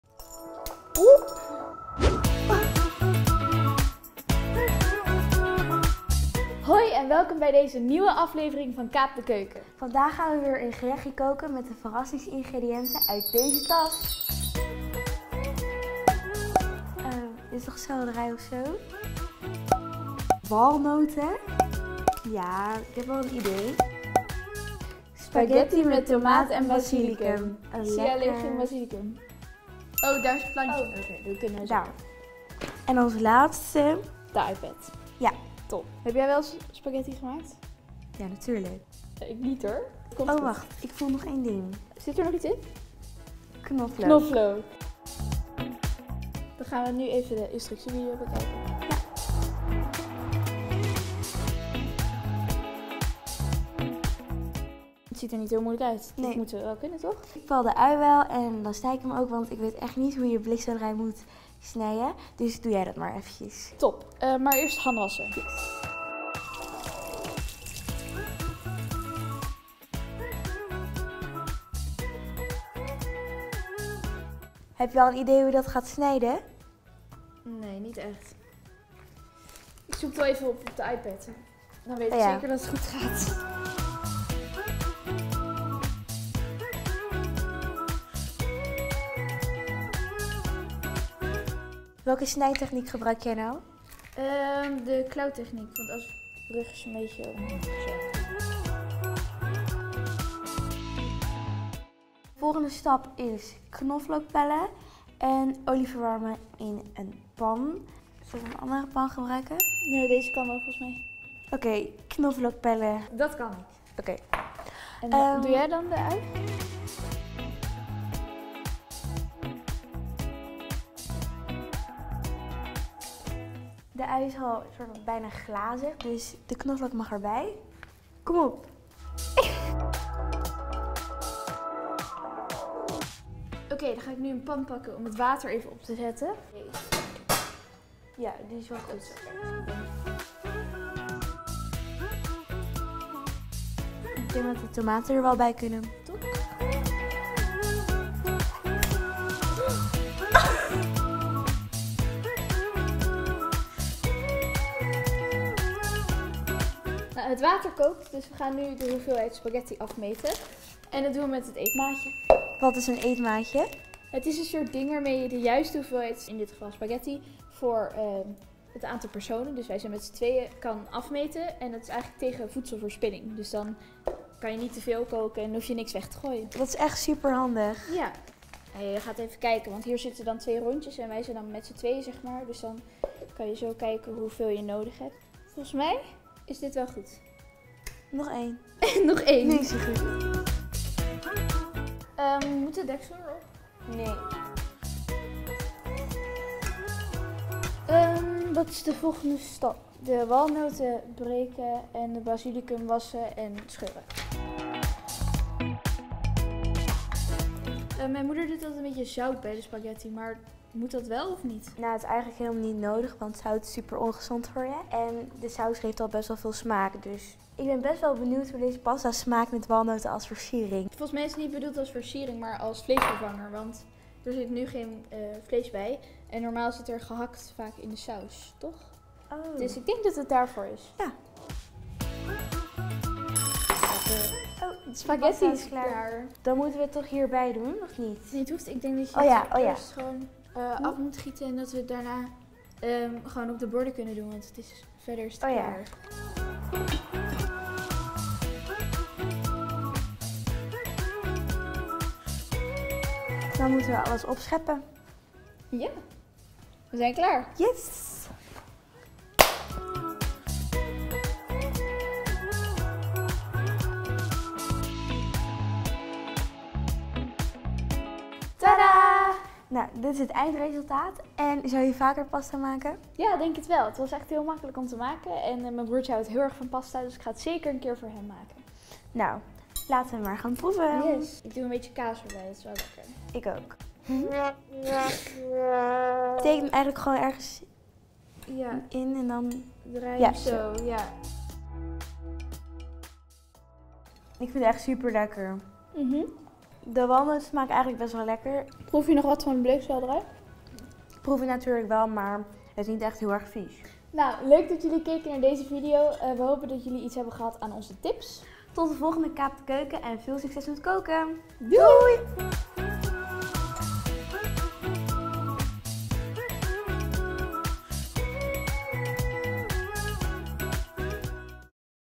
Spaghetti met tomaat en basilicum | Kaap de Keuken #2 | Weet wat je eet
Spaghetti, tomaat & basilicum maken - In de serie Kaap de Keuken! gaan Tessa en River koken met de ingrediënten in de verrassingstas. Gelukkig zit er ook een recept in de tas! Zo, dat blijkt mee te vallen. Ben jij klaar om thuis de keuken een keer te kapen?

De serie Kaap de keuken! is onderdeel van de praktische les van Weet wat je eet. Het enige complete lespakket over voeding: gezond, duurzaam en veilig eten. Gratis beschikbaar voor vmbo, havo, vwo en mbo.